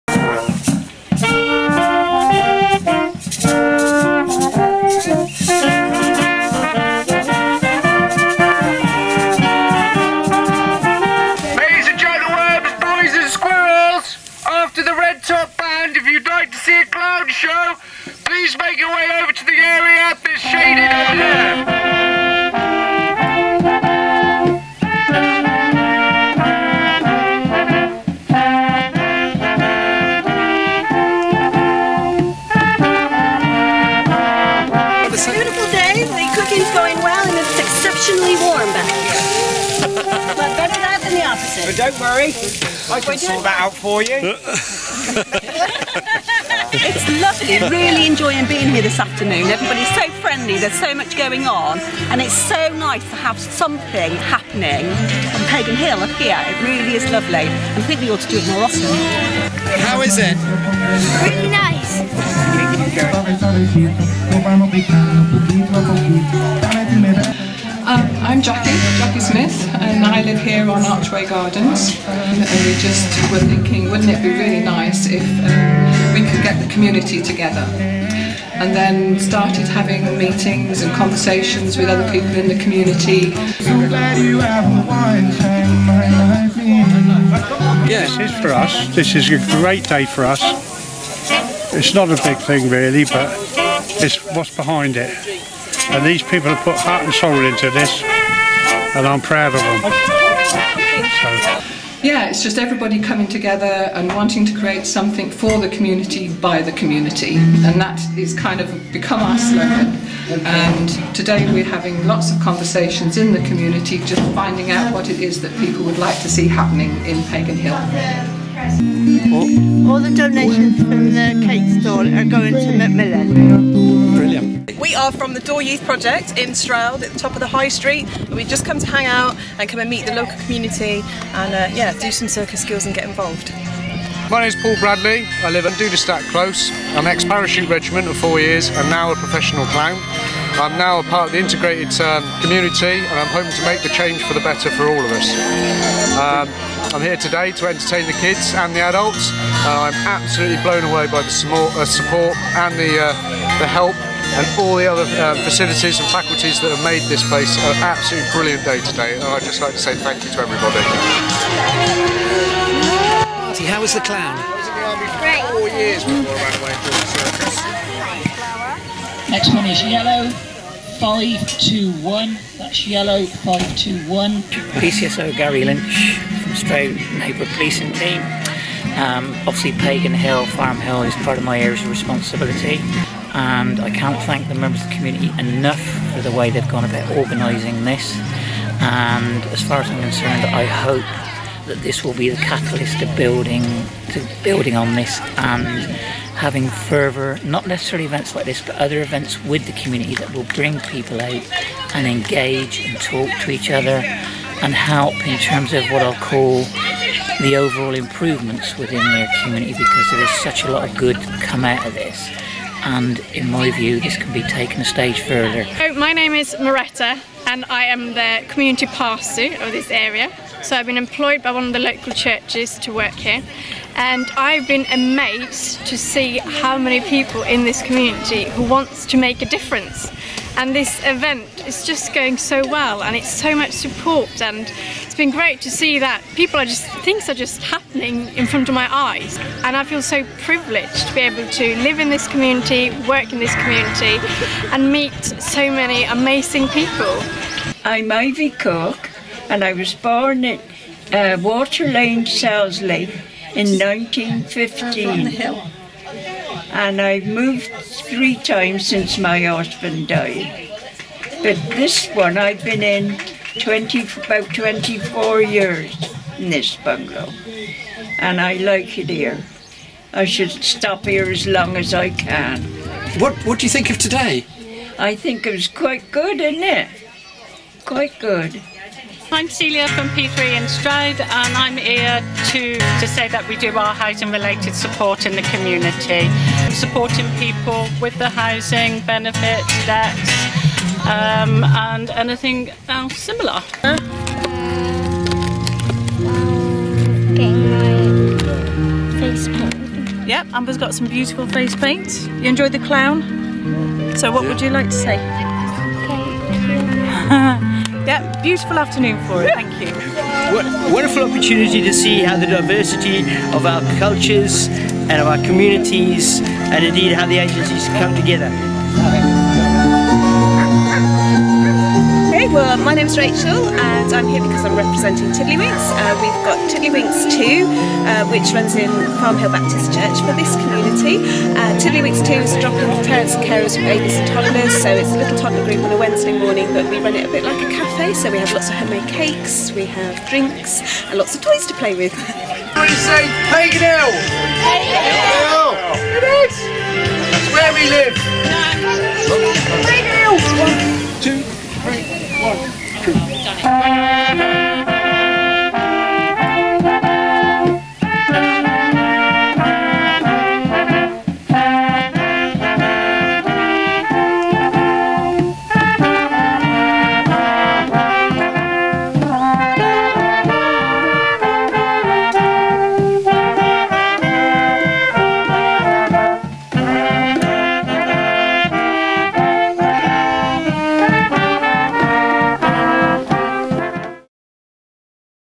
Paganhill Community Street Party 2017
Paganhill Community Street Party 2017 - Saturday 17th June at Archway Gardens; a community party organised by residents who are hoping this will lead to more conversations about what can happen in the neighbourhood.

I can't believe we missed Jacqueline Smith off teh credits - she was one of the key organisers!! Film thrown together by Philip Booth, whose family live in the area; big thanks to all who agreed to be in the film and apologies to those  missed.